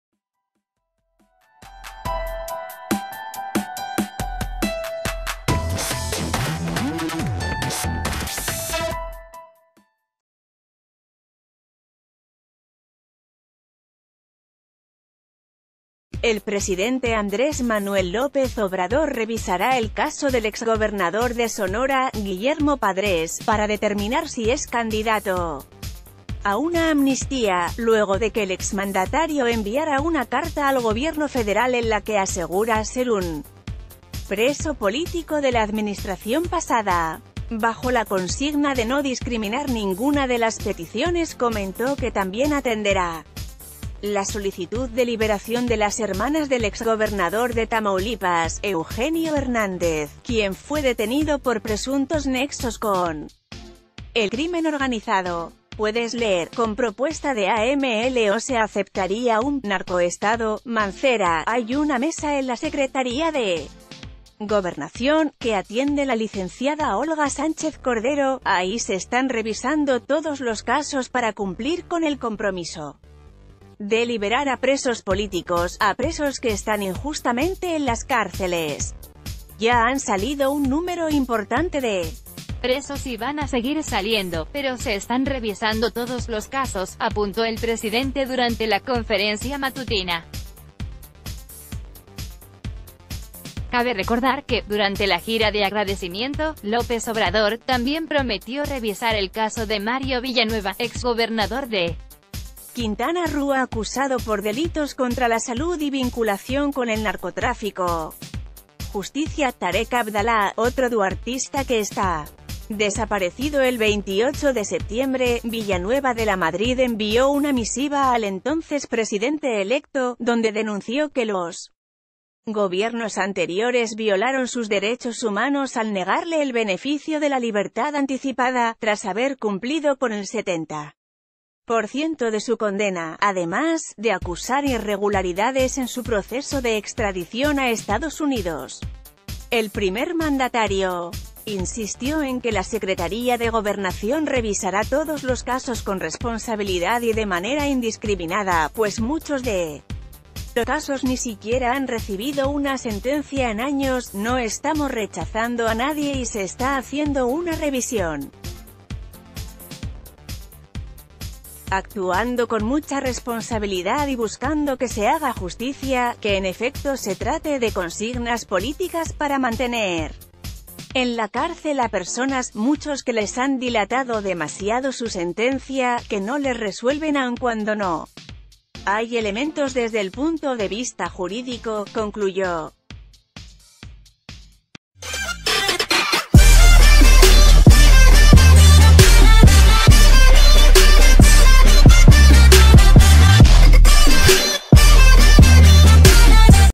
El caso de Padrés será considerado para amnistía: AMLO
El presidente Andrés Manuel López Obrador revisará el caso del exgobernador de Sonora, Guillermo Padrés, para determinar si es candidato a una amnistía, luego de que el exmandatario enviará una carta al gobierno federal en la que asegura ser un preso político de la administración pasada. Bajo la con...